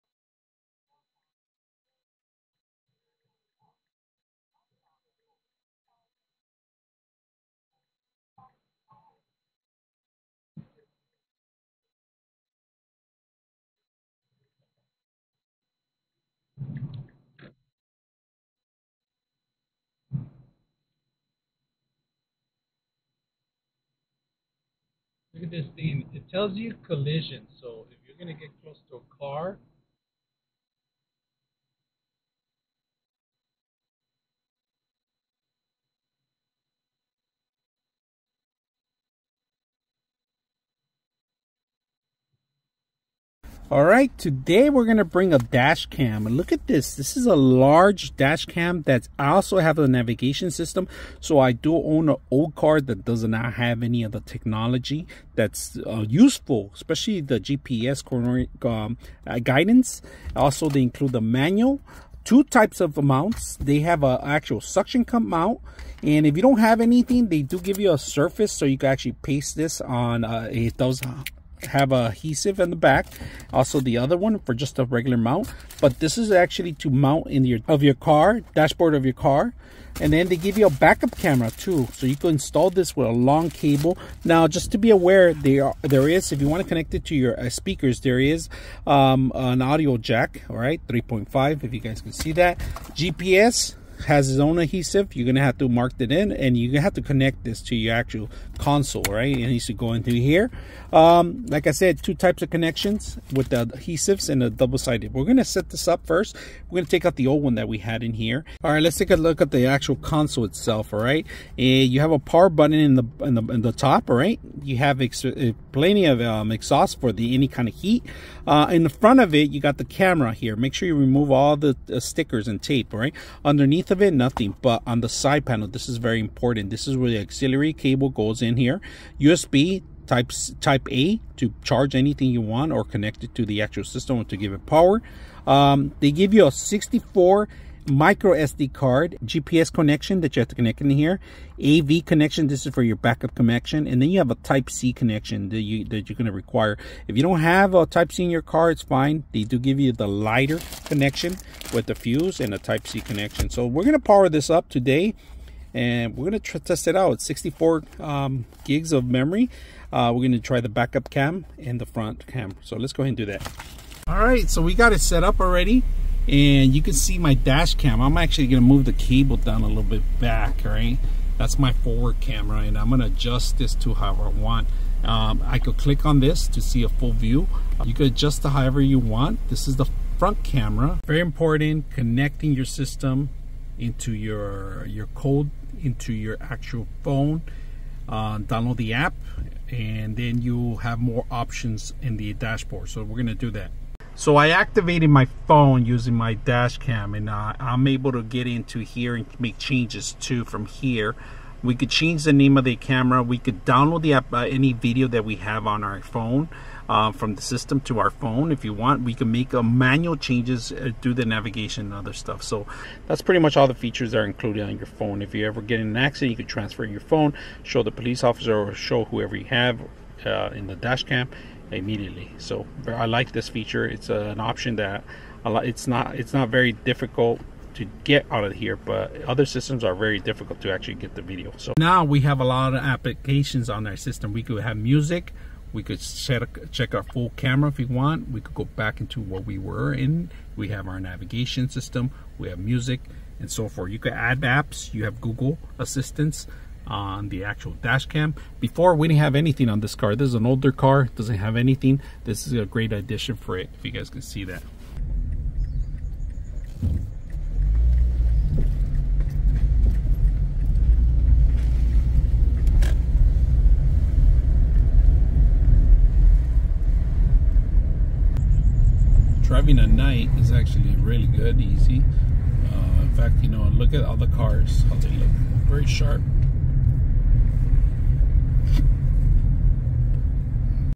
Wireless Apple Carplay & Android Auto
Wireless Apple Carplay & Android Auto, 9.3 Portable Car Stereo with 4K ADAS Dash Cam, Phone Mirroring, AUX/FM/Speaker/Bluetooth, 32.8ft Backup Camera,Voice Control . Thank You